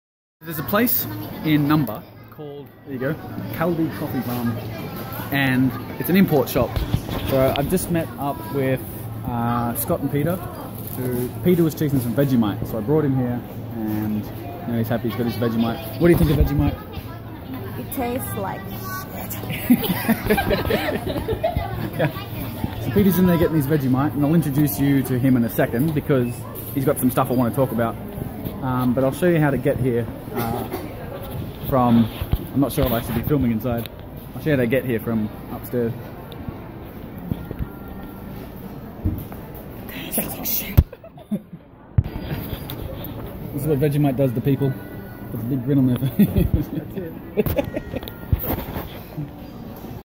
New info (1/2) Import shop, Namba
I hate video editing with a passion so I didn't put the two together.

Links to shops below.
･Import shop in Namba is Kaldi. B1 of the ○一○一デパート department store. "Marui departo" (pronounced "maru-ee depaa-toh") Walk in, go round the back to the escalator going down and then swing a right.

･I can never remember what the one in Umeda is called. It's a white shop with maroon writing. The name is written in kanji in a funky font. It's in the Umeda underground stretch of shops called "Whity" which is written in English up on the main street. Take any set of stairs in the area to get down there. It's a bit maze-y but once you've got your bearings you'll be right. If you can see Yoshinoya, you're super close. It's on the opposite side and a few stores heading away from the fountain.

･Google Maps links:
Kaldi, Namba: